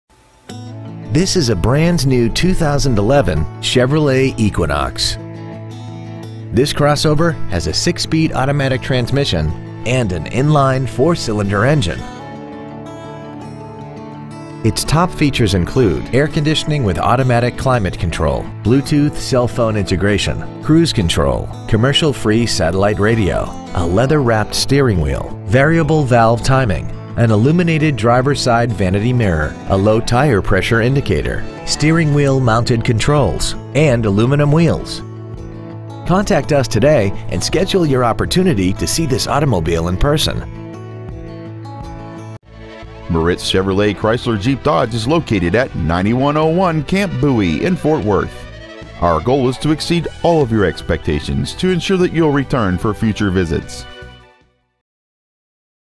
2011 Chevrolet Equinox Ft. Worth TX
Year: 2011
 Make: Chevrolet
 Model: Equinox
 Engine: Gas I4 ECOTEC 2.4/146.5
 Trans.: Automatic

 2011 Chevrolet Equinox Ft Worth TX